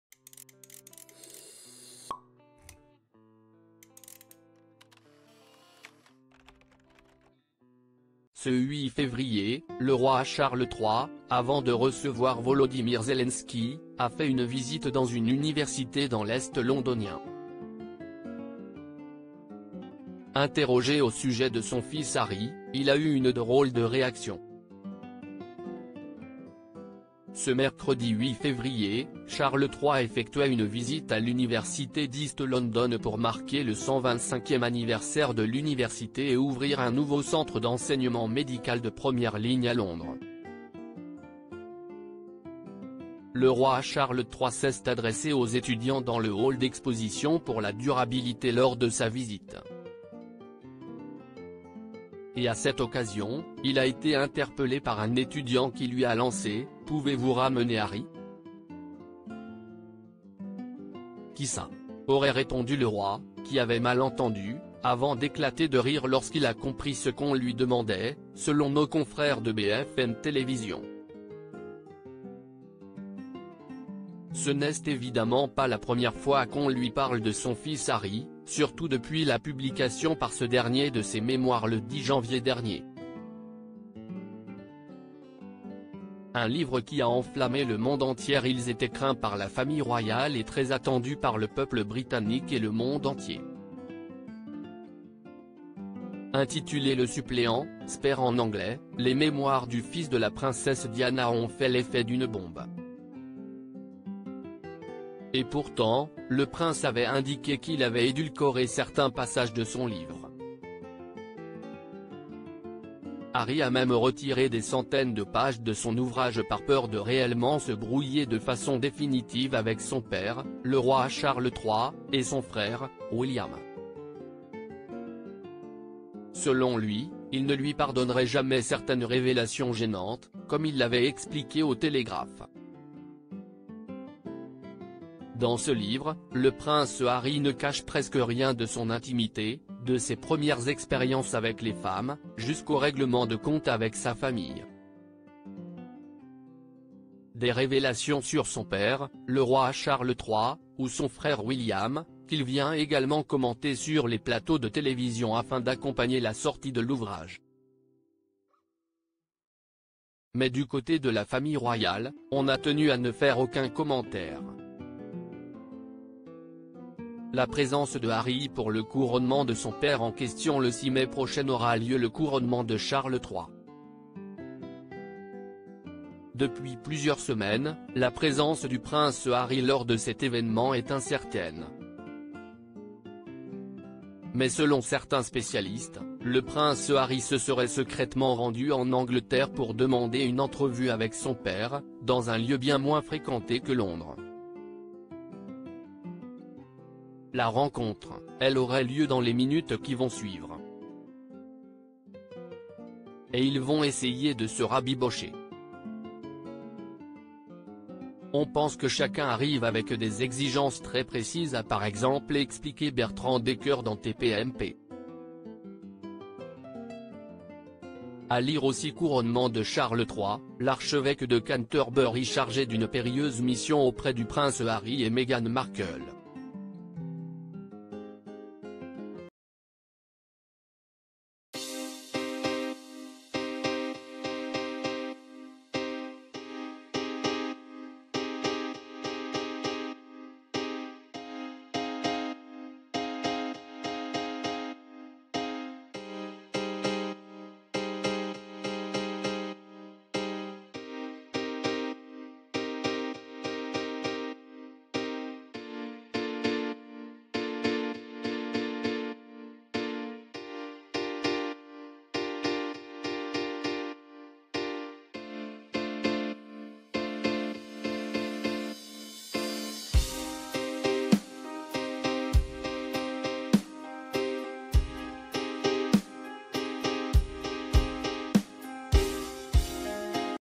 "Qui ça ?" : la drôle de réaction du roi Charles III, interrogé sur un retour possible de son fils H
"Qui ça ?" : la drôle de réaction du roi Charles III, interrogé sur un retour possible de son fils Harry pour son couronnement